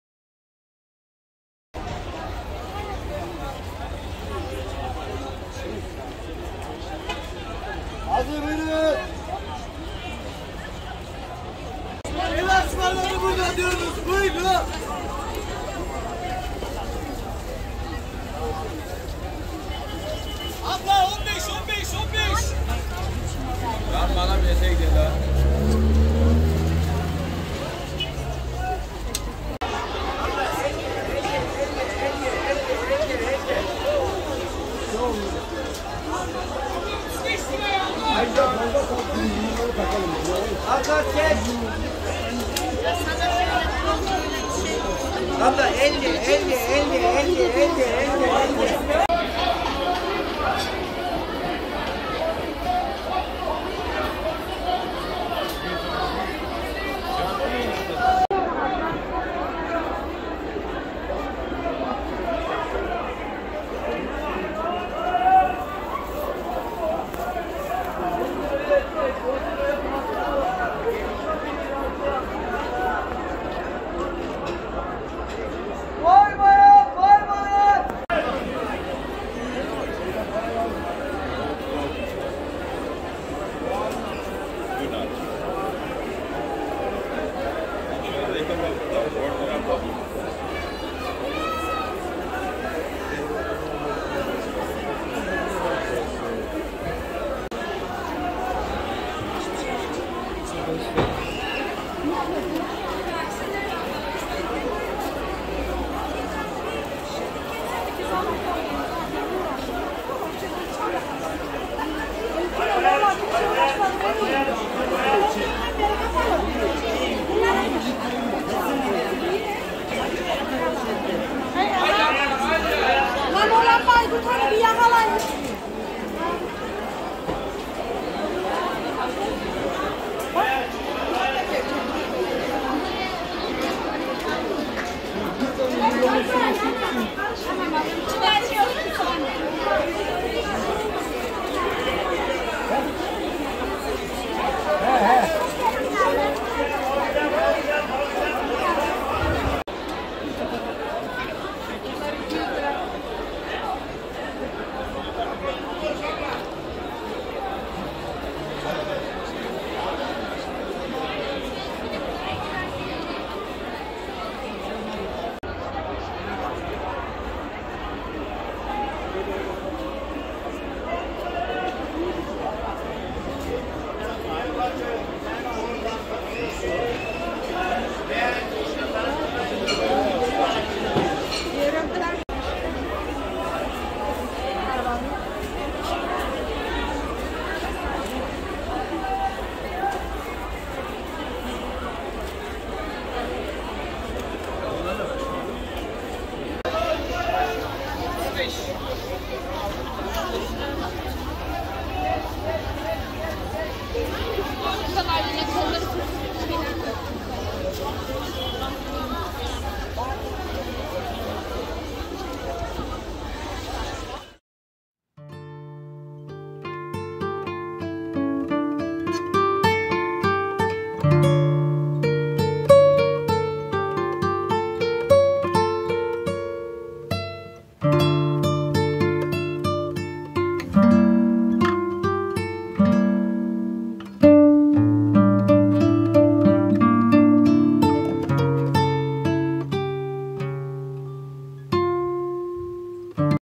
Thursday market in Ankara | A weekly market | Turkiye | 2022
This market is located in the central part of Ankara city in Çankaya district.And it is held on Thursdays.
All kinds of goods are offered to customers at very low prices.
# local market